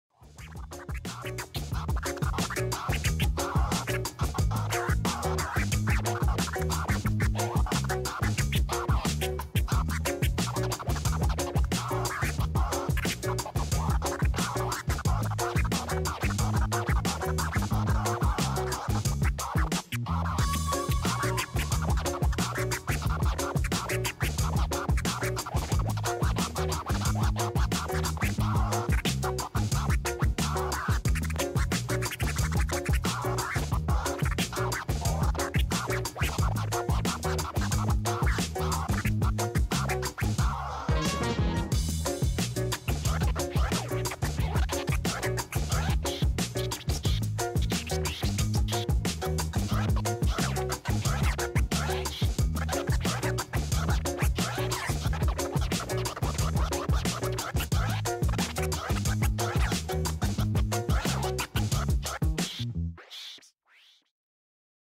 DJ Bobby Kut - Ortofon Skratch Nerds World Battle (2019) - ROUND 1
Ortofon Skratch Nerds on-line World Scratch Battle 2019 powered by Ortofon